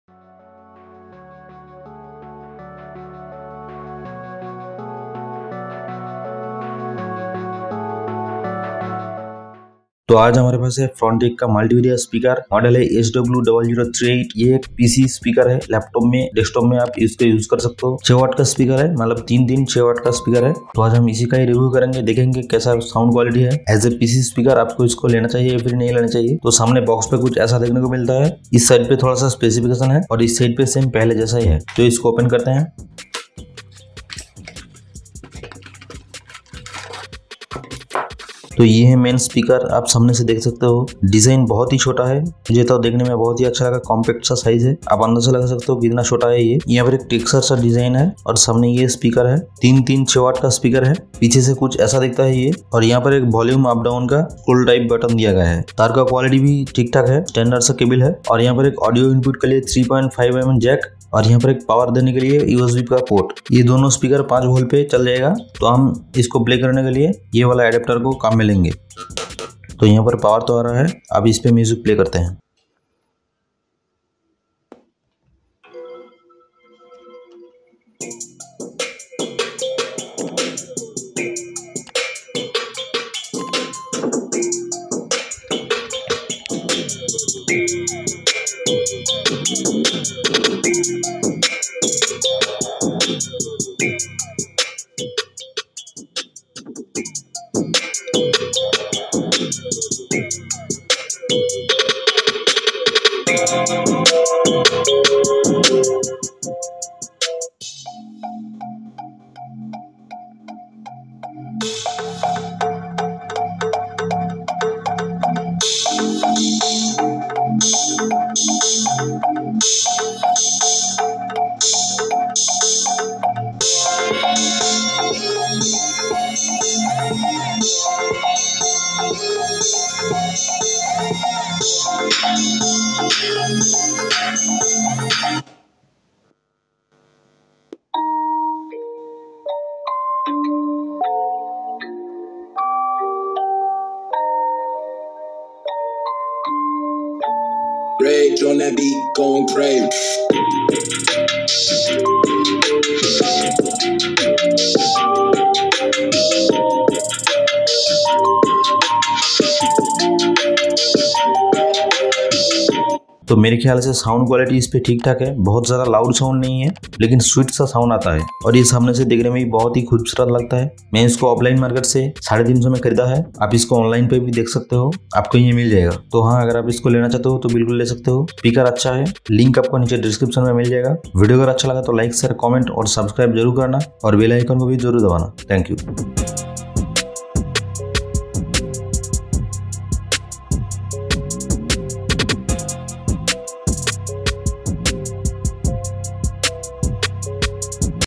frontech speaker review | frontech SW 0038 | best pc speaker under 500 | frontech multimedia speaker
Here we have reviewed a PC speaker frontech sw-0038. It has a very good design and speaker sound quality is very good.

frontech speaker review | frontech speaker SW 0038 | best pc speaker under 500 | frontech multimedia speaker